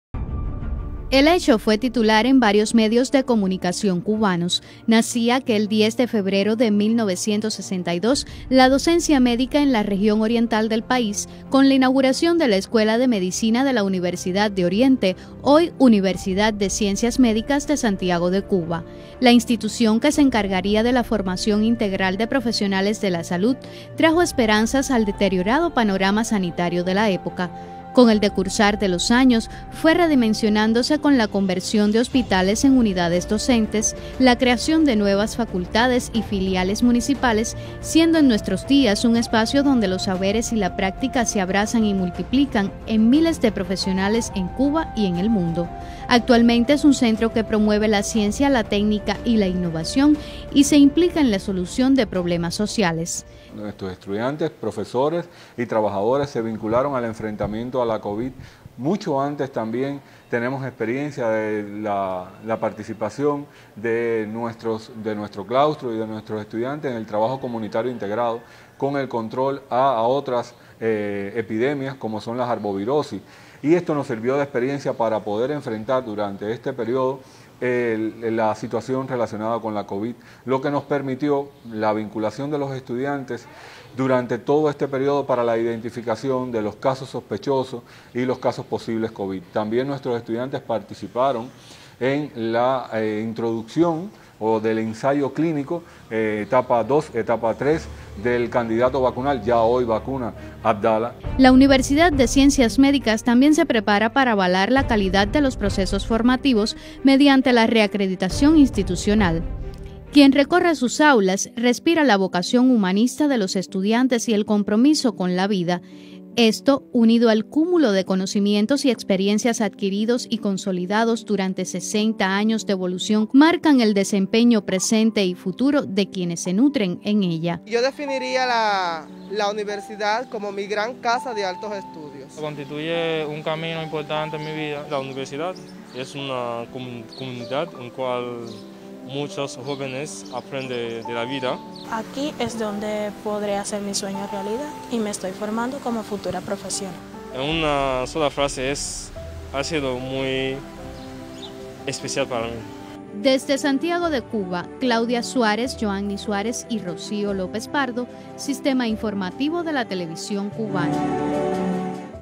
Inicio de la docencia médica en Santiago de Cuba
El 10 de febrero de 1962 es la fecha que marca el inicio de la docencia médica en la región oriental tras ser inaugurada la escuela de medicina de la Universidad de Oriente, hoy Universidad de Ciencias Médicas de Santiago de Cuba. Durante estos sesenta años la formación de profesionales de la salud ha venido consolidándose siendo cada día mas humanista y con amplio alcance territorial.